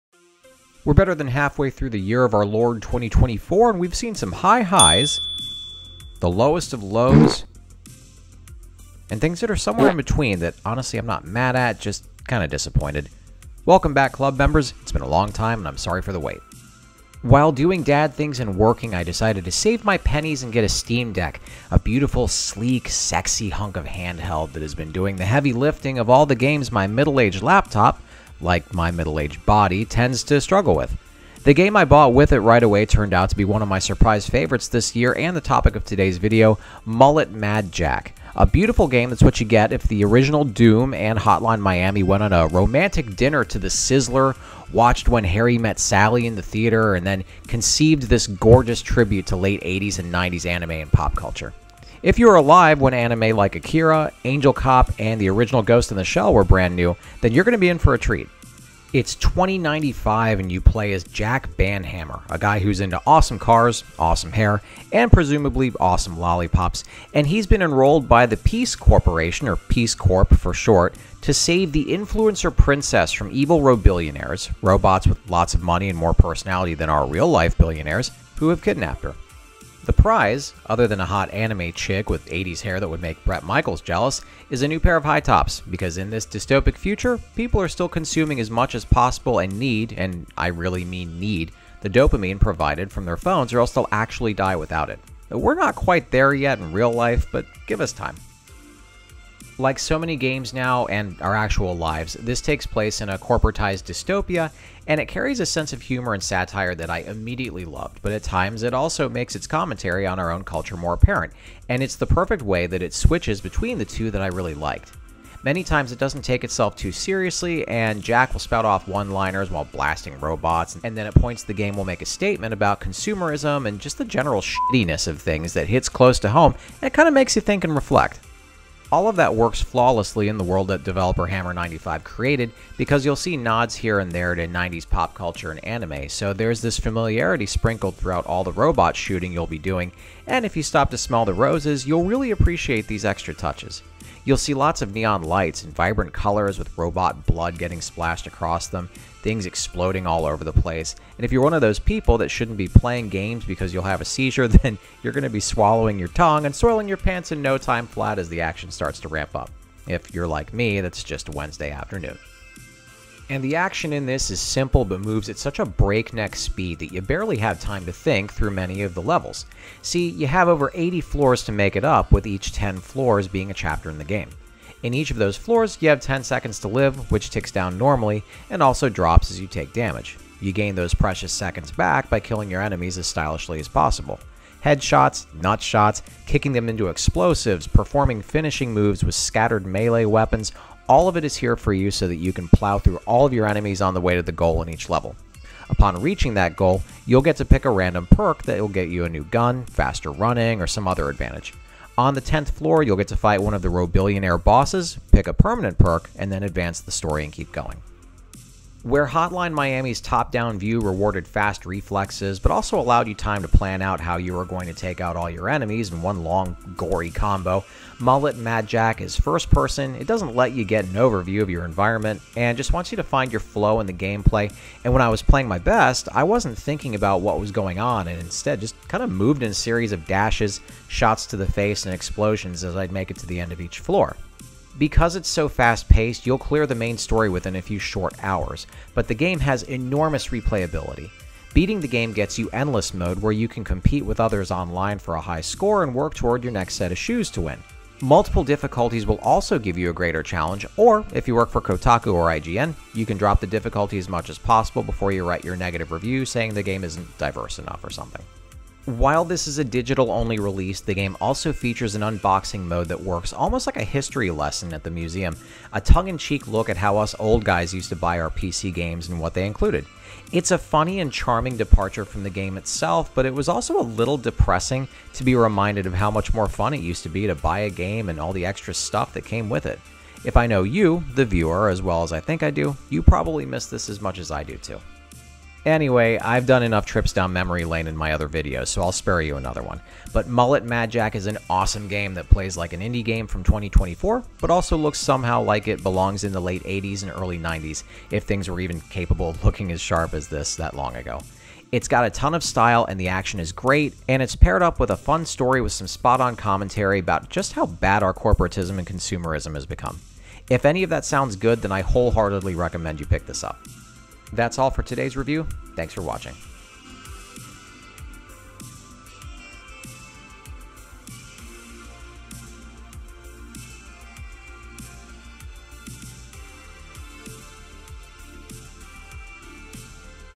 Mullet Madjack (Steam) Review: Party In The Front, Party In The Back
In a dystopian (and remarkably familiar) future where the dopamine we get from our entertainment actually keeps us alive, Jack Banhammer has to rescue the influencer (ugh) and win a new pair of shoes (yay!).  This is an awesome game all around - check out the review to find out why.